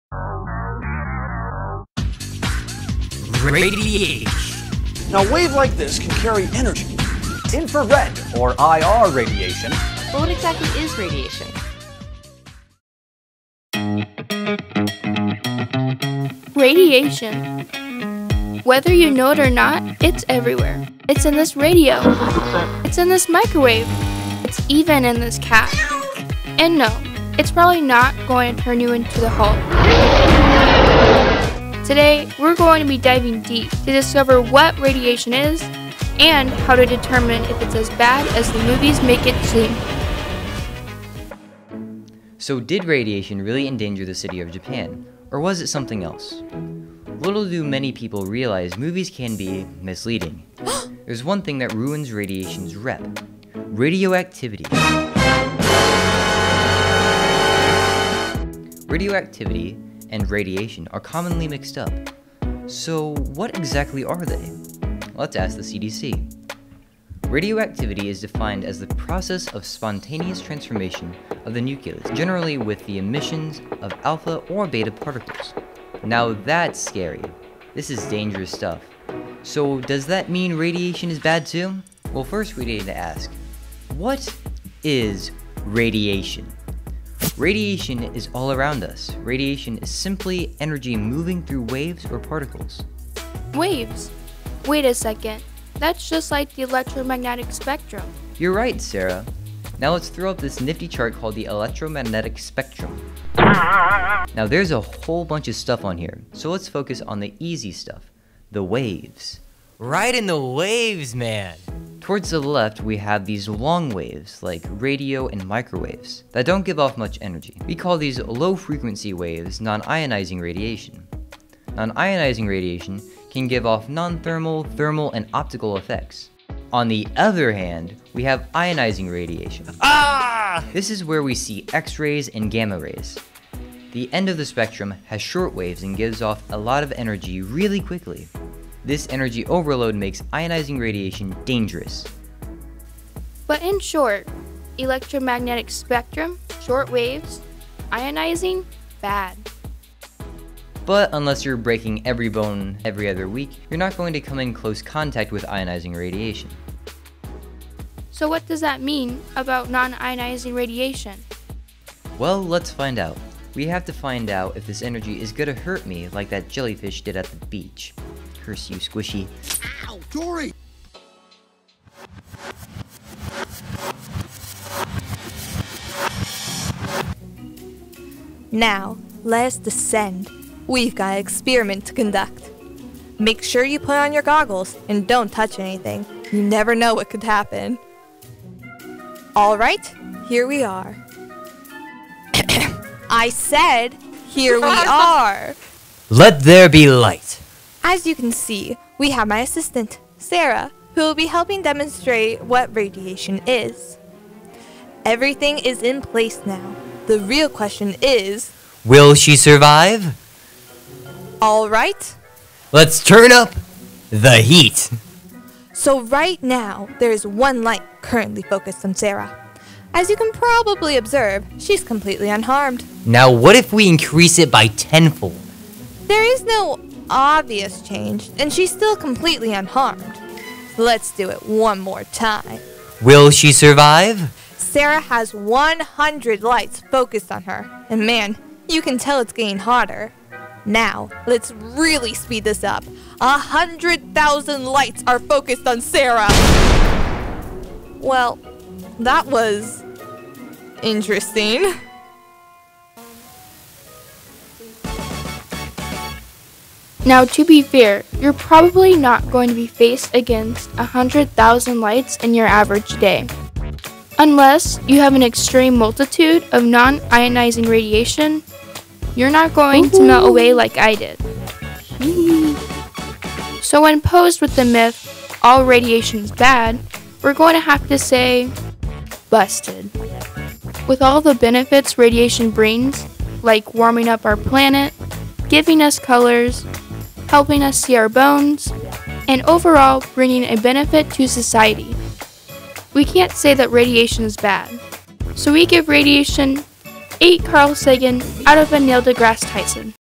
CCHS Science Class - Chem Radiation
The students of CCHS in Mr. Strass' science classes were asked to evaluate a statement about radiation. We're proud of their presentation they created. Student researched, scripted, filmed, performed, and edited this clip!